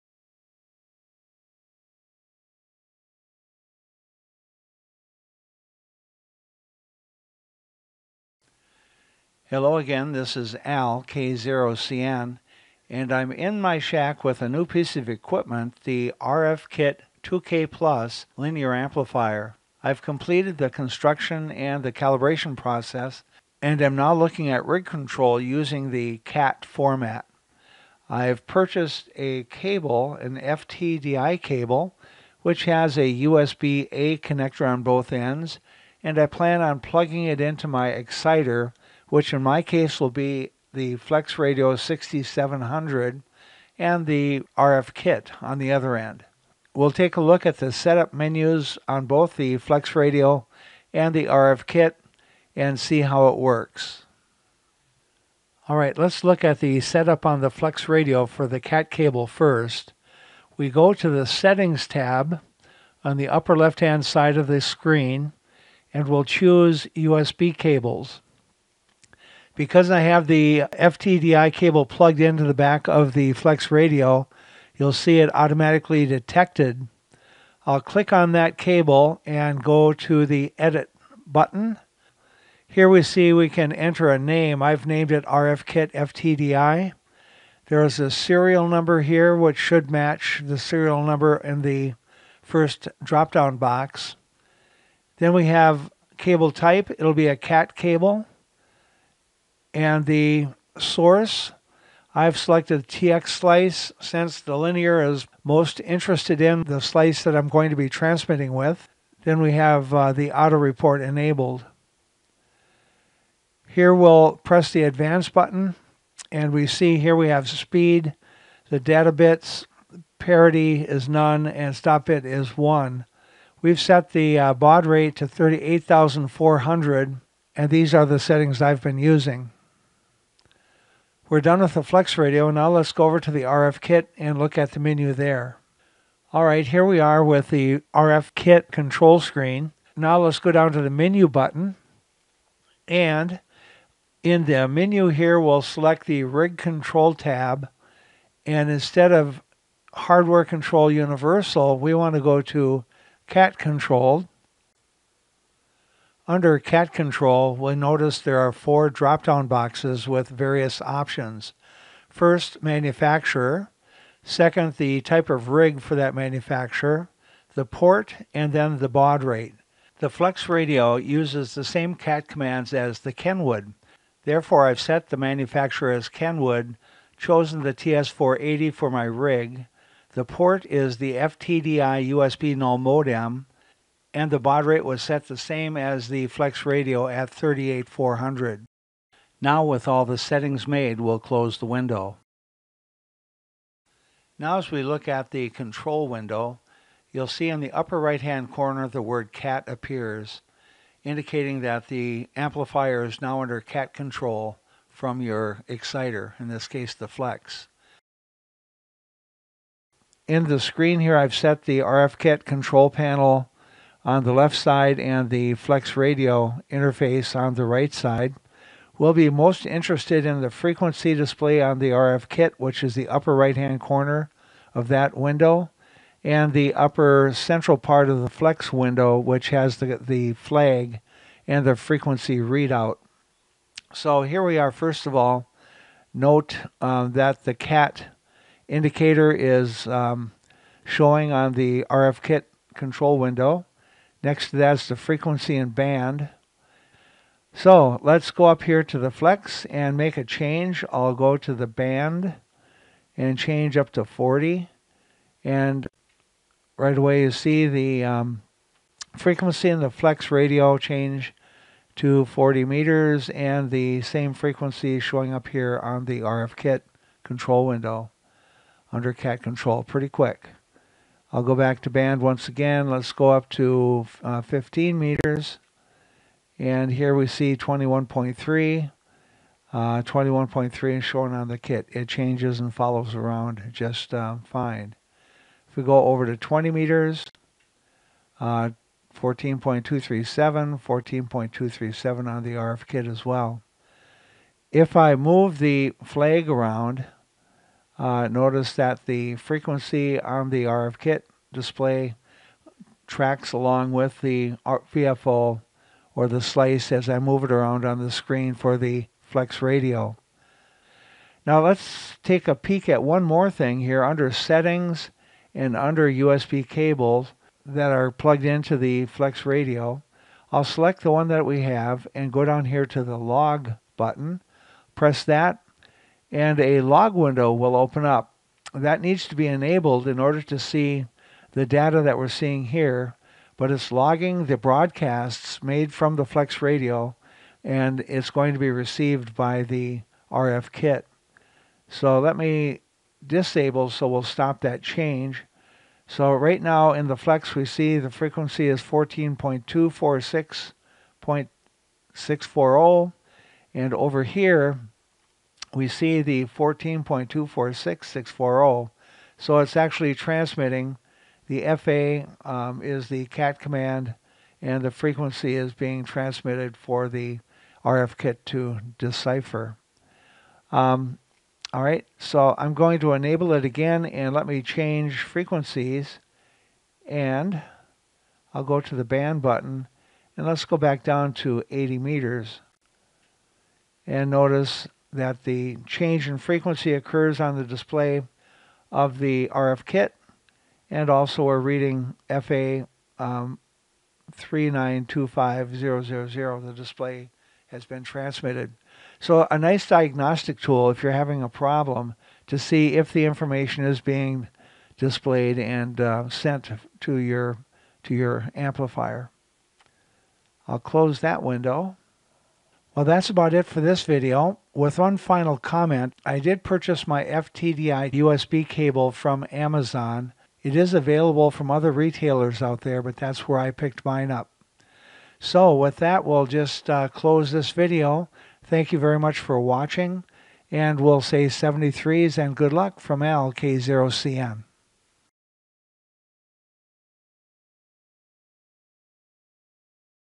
B26 RF-2K+ CAT control with FlexRadio
CAT control of the B26 RF2K+ amplifier with the Flex 6000 series radios. Using an FTDI Chip, USB cable, this video walks through settings for both the Flex 6700 running SSDR v. 3.27 and the B26 RF-2K+ linear amplifier. CAT control uses frequency data broadcast from the FlexRadio which allows the amplifier to automatically change bands and frequency, matching the exciter. This is an important feature when using the auto antenna tuner, built into the RF2K+.  The USB cable is available from Amazon and other vendors.